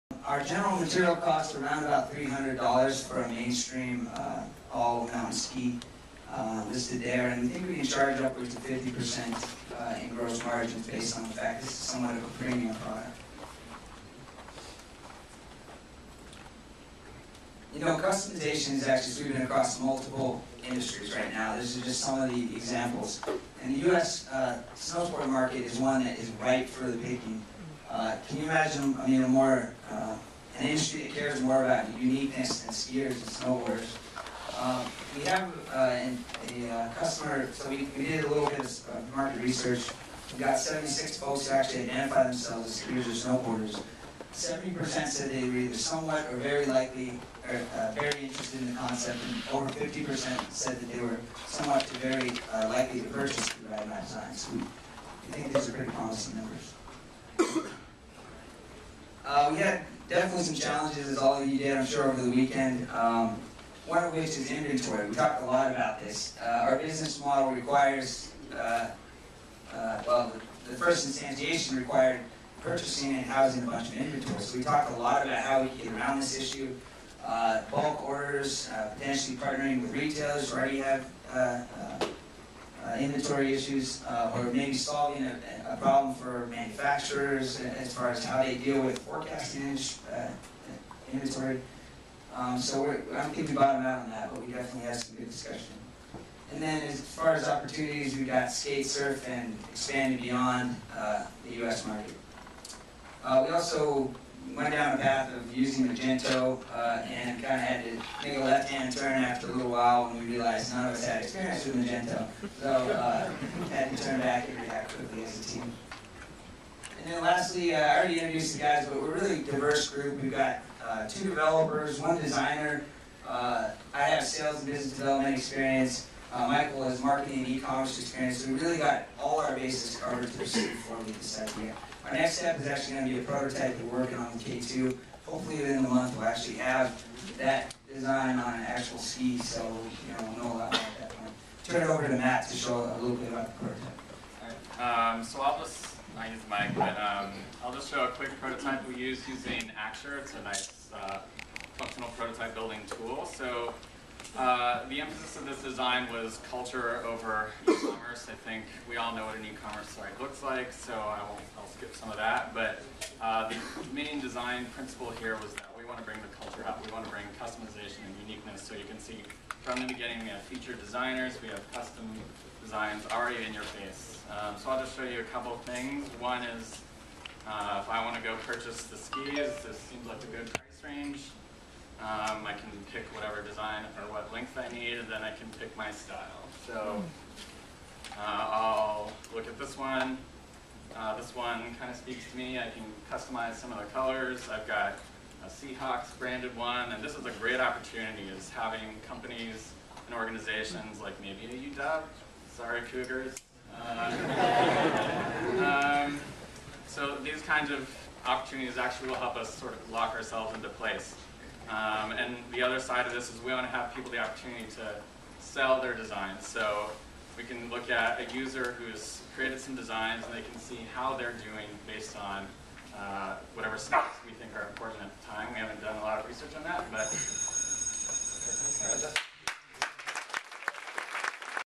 Ride My Design at Seattle Startup Weekend
Ride My Design presents at Seattle Startup Weekend 2011. Sorry for the partial video, the other camera was supposed to film this presentation, but the battery on it died halfway